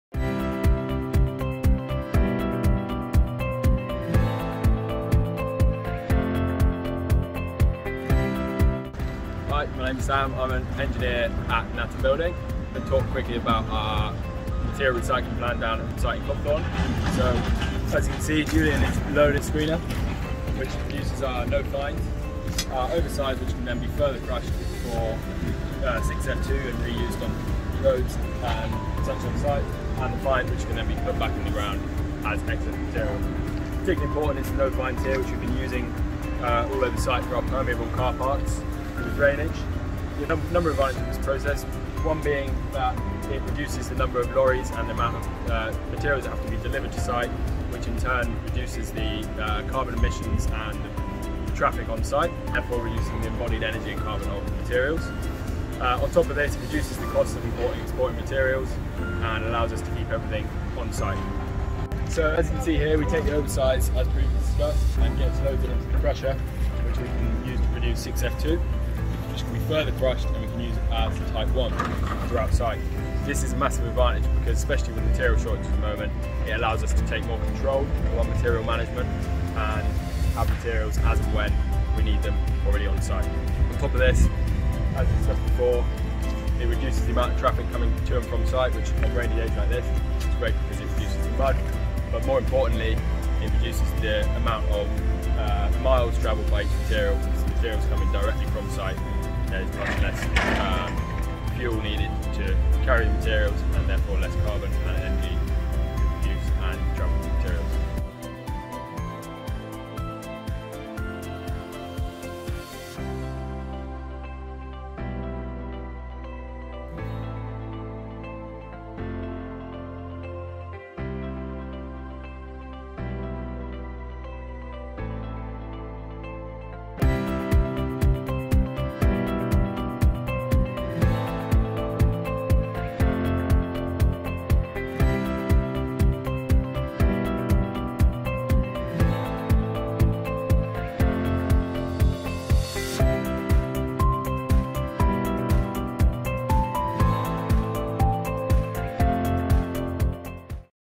Copthorne Recycling Film
Showing how Crushers and Screeners can help reduce the carbon footprint on a construction project.